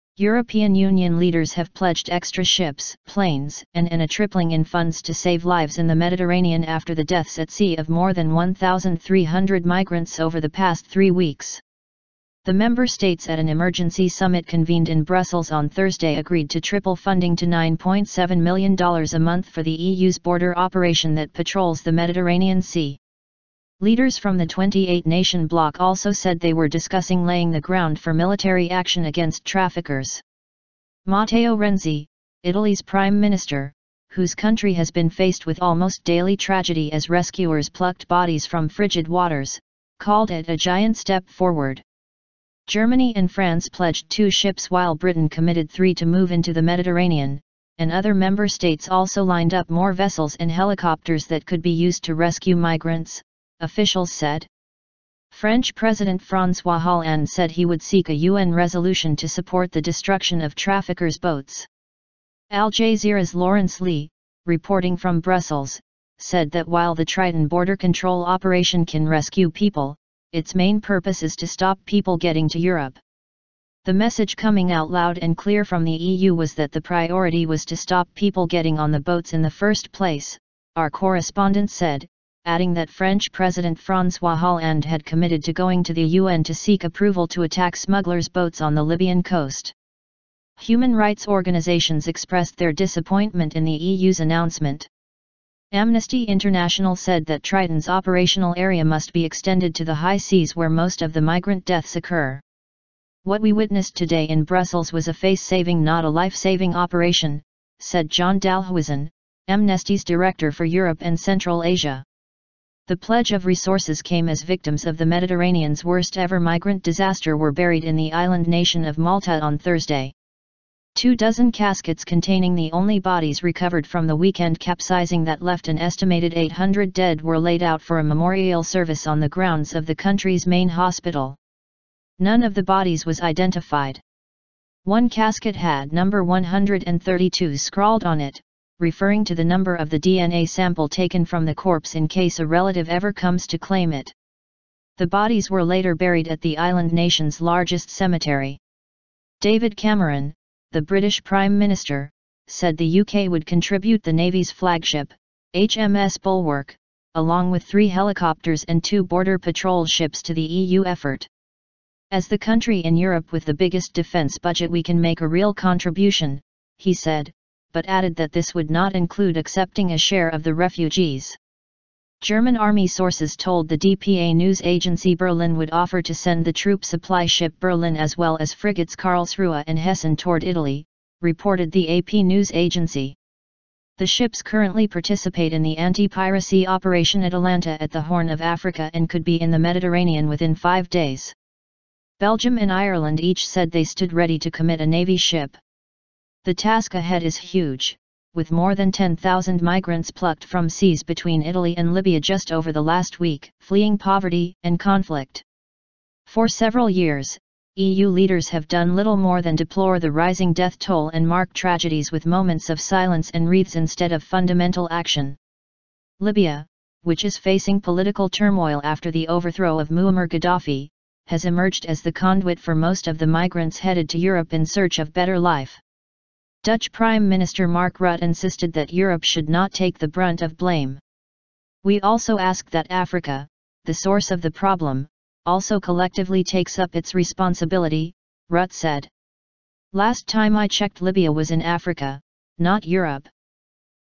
EU triples fund to address migrant crisis
European Union leaders have pledged extra ships, planes and and a tripling in funds to save lives in the Mediterranean after the deaths at sea of more than 1,300 migrants over the past three weeks.
The member states at an emergency summit convened in Brussels on Thursday agreed to triple funding to $9.7m a month for the EU's border operation that patrols the Mediterranean Sea.
Leaders from the 28-nation bloc also said they were discussing laying the ground for military action against traffickers.
Matteo Renzi, Italy's prime minister, whose country has been faced with almost daily tragedy as rescuers plucked bodies from frigid waters, called it "a giant step forward".
Germany and France pledged two ships while Britain committed three to move into the Mediterranean, and other member states also lined up more vessels and helicopters that could be used to rescue migrants, officials said.
French President Francois Hollande said he would seek a UN resolution to support the destruction of traffickers' boats.

Al Jazeera's Laurence Lee, reporting from Brussels, said that while the Triton border-control operation can rescue people, its main purpose is to stop people getting to Europe.
"The message coming out loud and clear from the EU was that the priority was to stop people getting on the boats in the first place," our correspondent said, adding that French President Francois Hollande had committed to going to the UN to seek approval to attack smugglers' boats on the Libyan coast.
Human rights organisations expressed their disappointment in the EU's announcement.
Amnesty International said that Triton's operational area must be extended to the "high seas" where most of the migrant deaths occur.

"What we witnessed today in Brussels was a face-saving not a life-saving operation," said John Dalhuisen, Amnesty's Director for Europe and Central Asia.
The pledge of resources came as victims of the Mediterranean's worst-ever migrant disaster were buried in the island nation of Malta on Thursday.
Two dozen caskets containing the only bodies recovered from the weekend capsizing that left an estimated 800 dead were laid out for a memorial service on the grounds of the country's main hospital.
None of the bodies was identified. One casket had "No 132" scrawled on it, referring to the number of the DNA sample taken from the corpse in case a relative ever comes to claim it.
The bodies were later buried at the island nation's largest cemetery.

David Cameron, the British prime minister, said the UK would contribute the navy's flagship, HMS Bulwark , along with three helicopters and two border patrol ships to the EU effort.
"As the country in Europe with the biggest defence budget we can make a real contribution," he said, but added that this would not include accepting a share of the refugees.
German army sources told the DPA news agency Berlin would offer to send the troop supply ship Berlin as well as frigates Karlsruhe and Hessen toward Italy, reported the AP news agency.

The ships currently participate in the anti-piracy operation Atalanta at the Horn of Africa and could be in the Mediterranean within five days.
Belgium and Ireland each said they stood ready to commit a navy ship.
The task ahead is huge, with more than 10,000 migrants plucked from seas between Italy and Libya just over the last week, fleeing poverty and conflict.
For several years, EU leaders have done little more than deplore the rising death toll and mark tragedies with moments of silence and wreaths instead of fundamental action.
Libya, which is facing political turmoil after the overthrow of Muammar Gaddafi, has emerged as the conduit for most of the migrants headed to Europe in search of better life.
Dutch Prime Minister Mark Rutte insisted that Europe should not take the brunt of blame.
"We also ask that Africa, the source of the problem, also collectively takes up its responsibility," Rutte said. "Last time I checked Libya was in Africa, not Europe."